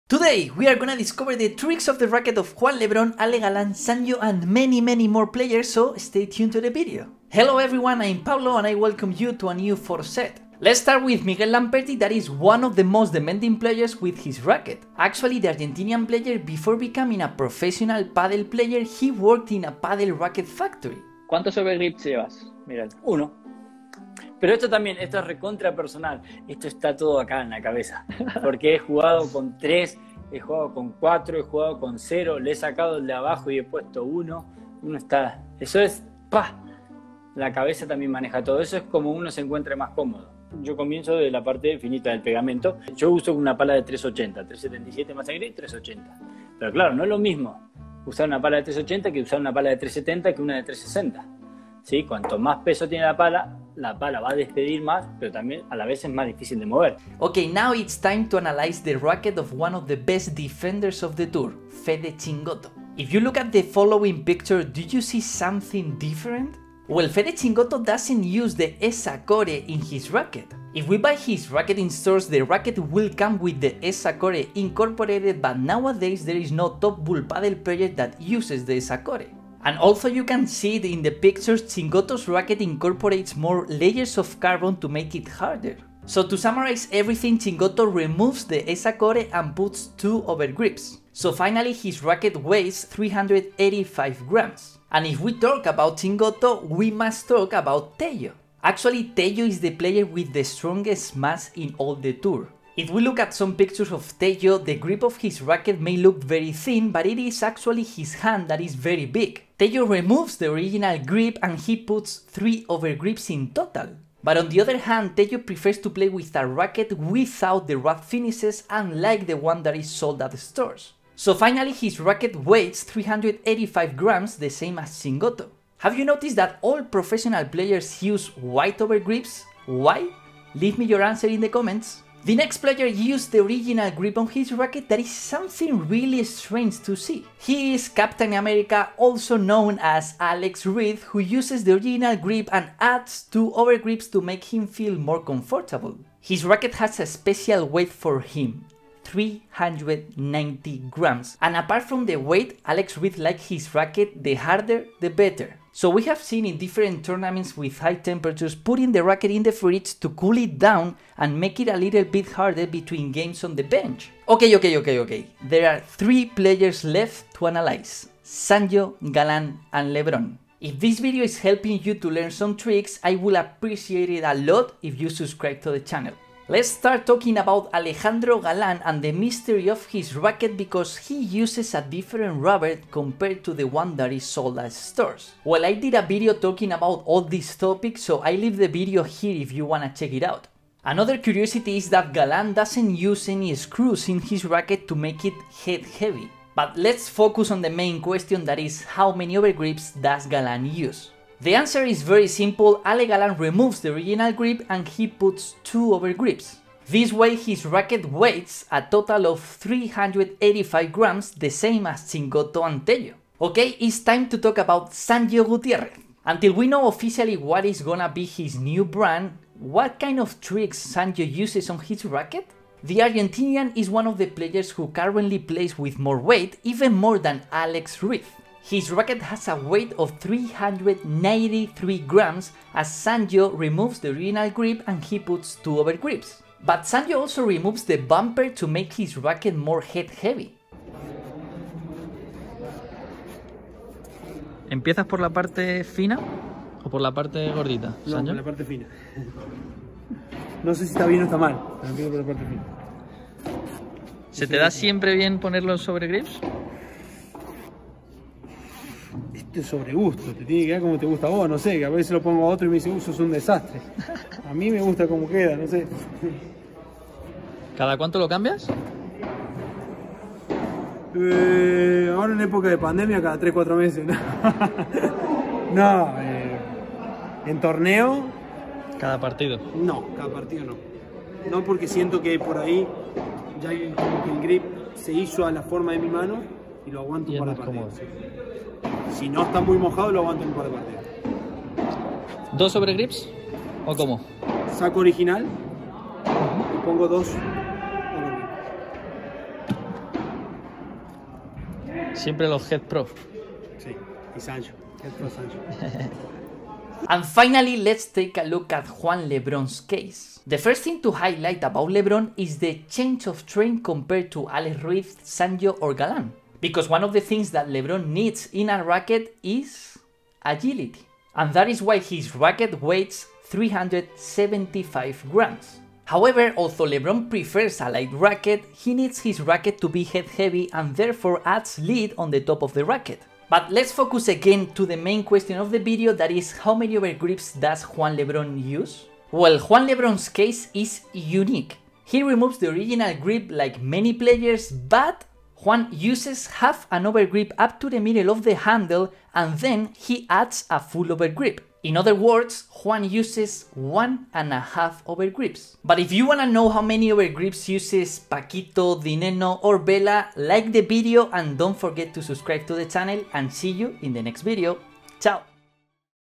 PADEL RACKET HACKS by JUAN LEBRON *GRIPS, WEIGHT...* - the4Set Padel
Juan LEBRON, the TOP 1 Padel player in the world, has a unique configuration of his racket. Do you wanna know how many overgrips Juan Lebron uses? And Sanyo?